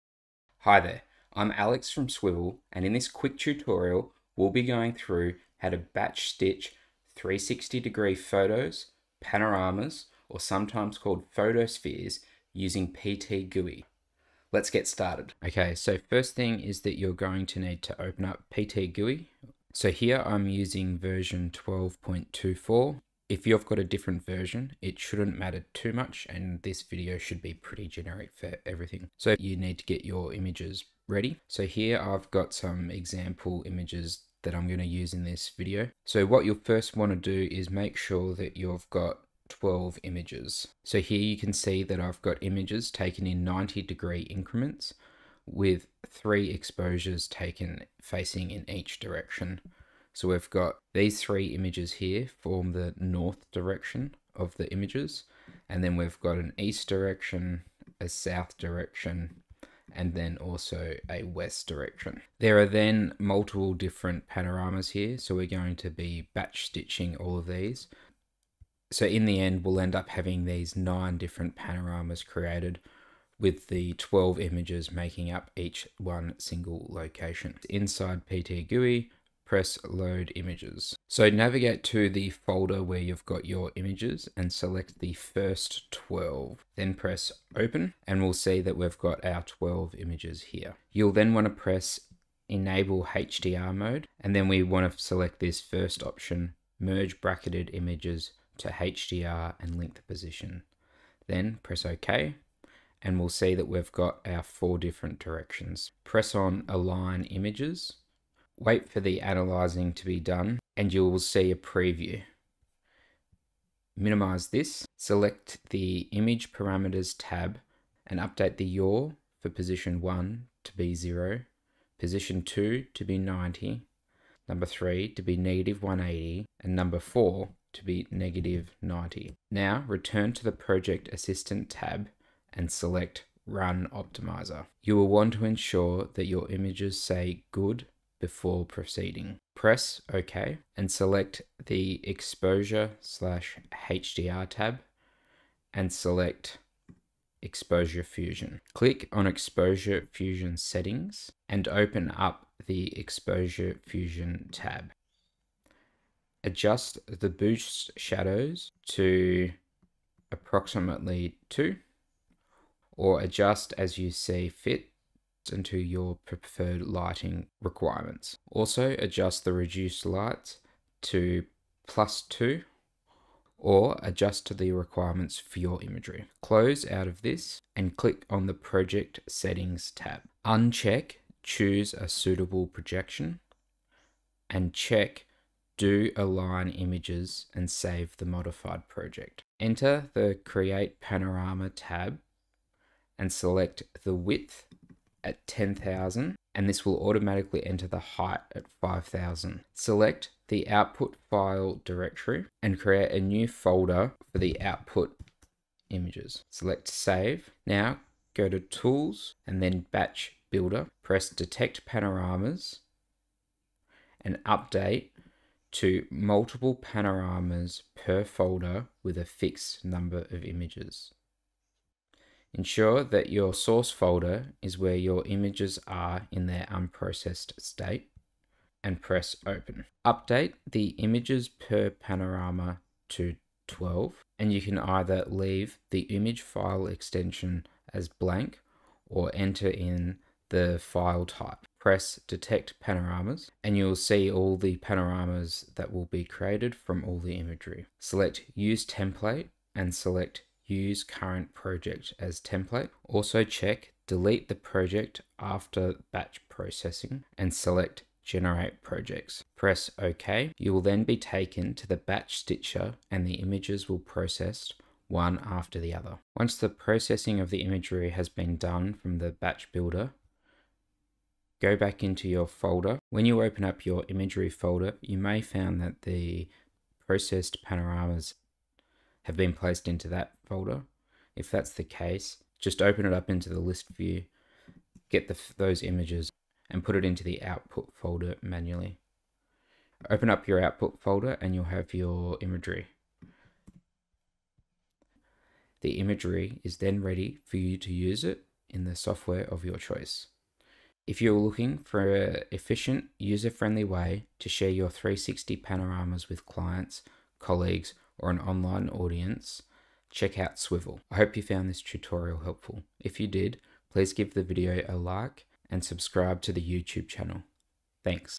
Batch Panorama Stitching in PTGui to create 360 Photos
**Please note that in order to use the Batch Builder you will need to be using PTGui Pro, the regular version does not support batch processing

Welcome from the Swyvl team! In this tutorial, Alex will show you how to use PTGui for batch panorama stitching to create stunning 360 photos. Perfect for virtual tours, real estate, and immersive experiences, this guide will help you streamline your workflow and achieve professional results.

In this video, you'll learn:

✅ How to set up PTGui for batch panorama stitching
✅ Tips and tricks for optimizing your 360 photo stitching process
✅ How to export high-quality panoramic images

Why PTGui?
PTGui is one of the top panorama stitching software for 360 photos, known for its precision and ease of use. Whether you're a beginner or a pro, PTGui offers the tools you need to create perfect panoramas.

Chapters:
0:00 Introduction
0:29 Get your files ready
1:08 Importing and Organizing Your Photos
2:20 Stitching Settings and Optimization
3:27 Setting Up PTGui for Batch Processing
4:55 Conclusion

Useful Links:

More Tutorials on 360 Photo Editing - coming soon